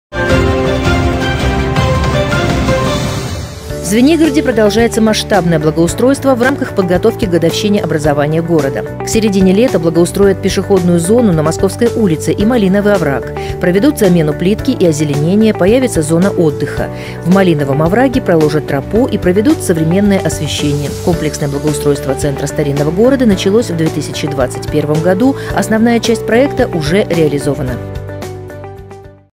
Малиновый овраг в Звенигороде благоустроят к середине лета
В Звенигороде продолжается масштабное благоустройство в рамках подготовки к годовщине образования города. К середине лета благоустроят пешеходную зону на Московской улице и Малиновый овраг.